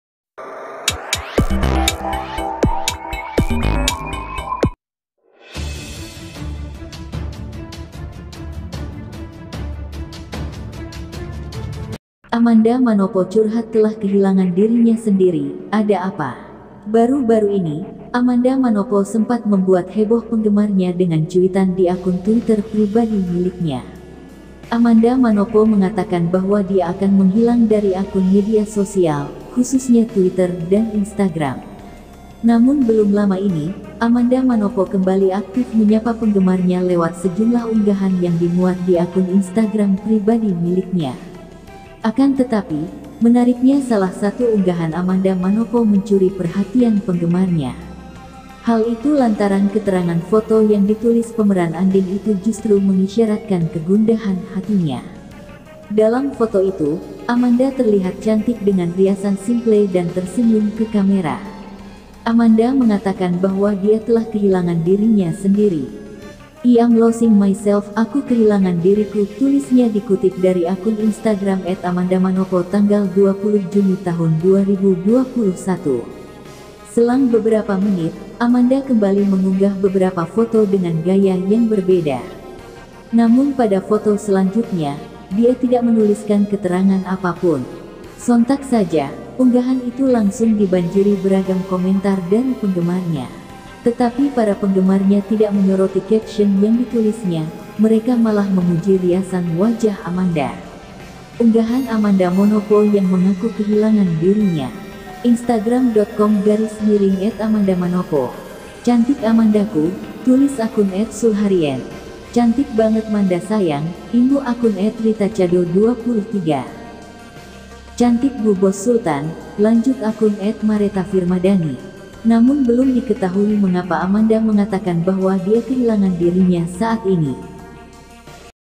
Amanda Manopo Curhat Telah Kehilangan Dirinya Sendiri, Ada Apa dengan dirinya..?
Baru-baru ini, Amanda Manopo sempat membuat heboh penggemarnya dengan cuitan di akun Twitter pribadi miliknya.

Namun belum lama ini, Amanda Manopo kembali aktif menyapa penggemarnya lewat sejumlah unggahan yang dimuat di akun Instagram pribadi miliknya.

Akan tetapi, menariknya salah satu unggahan Amanda Manopo mencuri perhatian penggemarnya.

Hal itu lantaran keterangan foto yang ditulis pemeran Andin itu justru mengisyaratkan kegundahan hatinya.

Dalam foto itu, Amanda terlihat cantik dengan riasan simple dan tersenyum ke kamera.

Amanda mengatakan bahwa dia telah kehilangan dirinya sendiri.